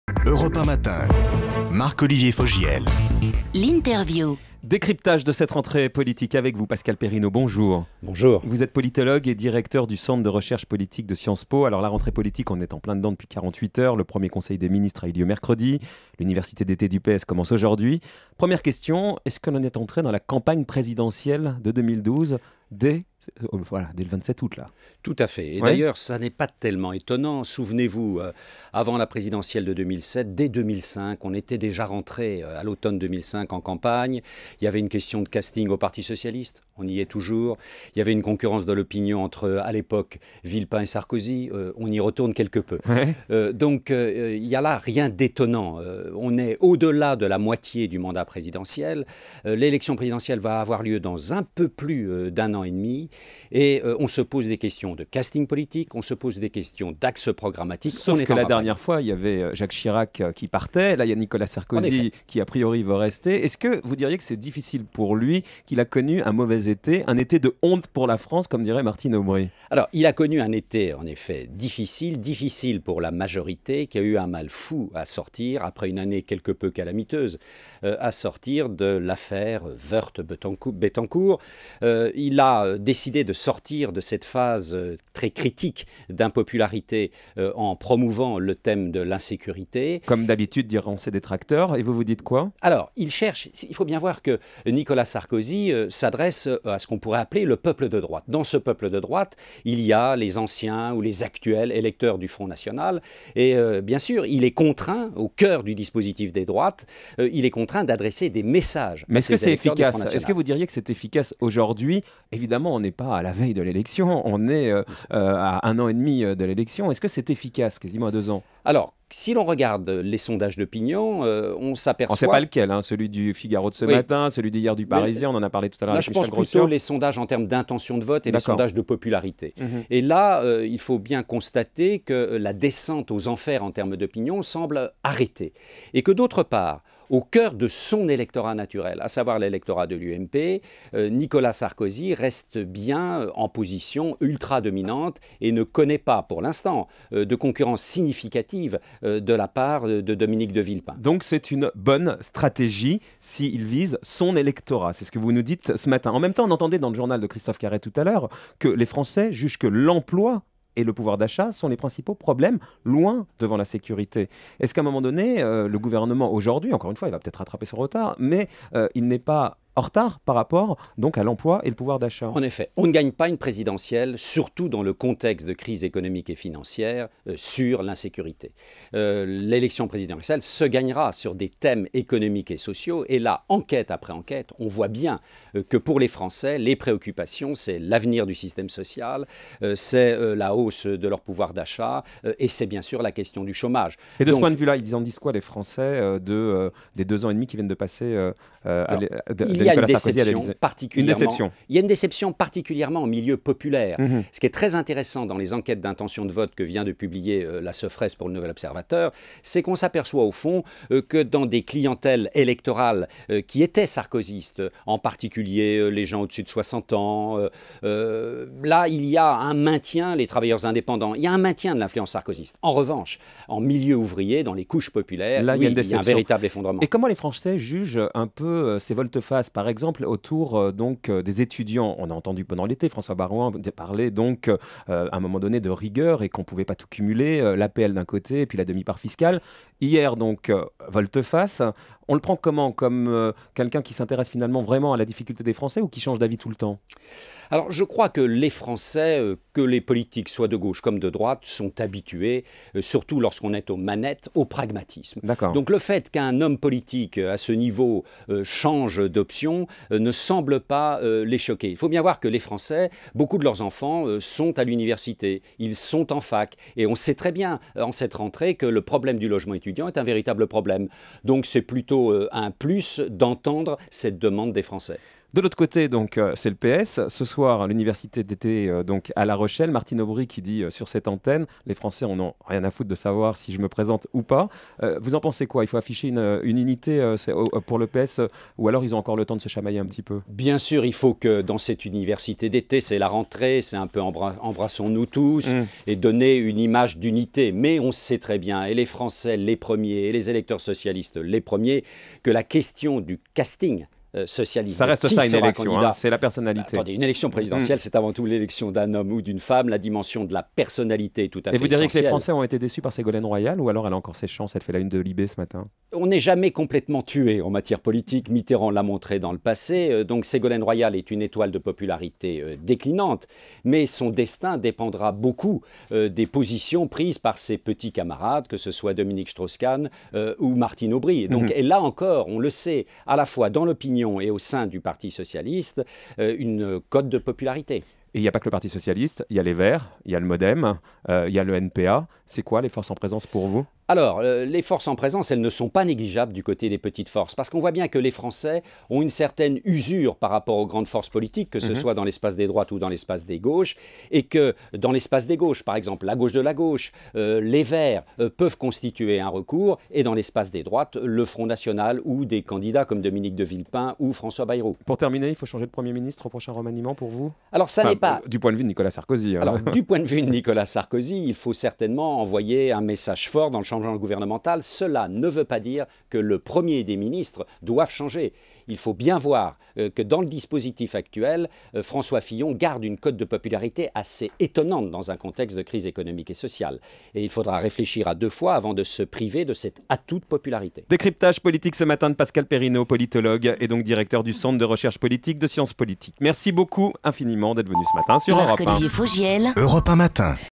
"La campagne pour 2012 a commencé"
Pascal Perrineau, directeur du CEVIPOF, analyse la situation politique de la rentrée.